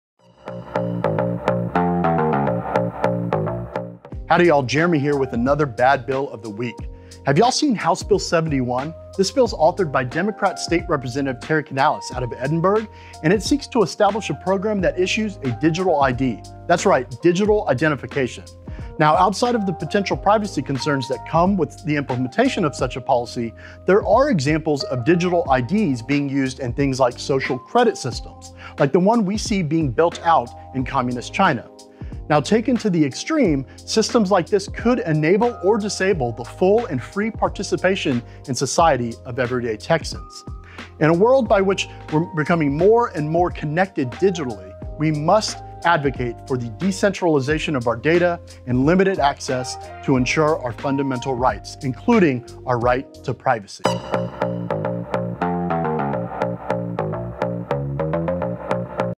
Bad Bill of the Week - House Bill 71
🚨 Continuing our weekly series throughout the duration of the 88th txlege session, 𝗕𝗔𝗗 𝗕𝗜𝗟𝗟 𝗢𝗙 𝗧𝗛𝗘 𝗪𝗘𝗘𝗞.

🔎 This week’s bill? House Bill 71 by Democrat State Rep. Terry Canales. The bill would attempt to implement a 'Digital I.D.' and comes with potential privacy concerns among other issues.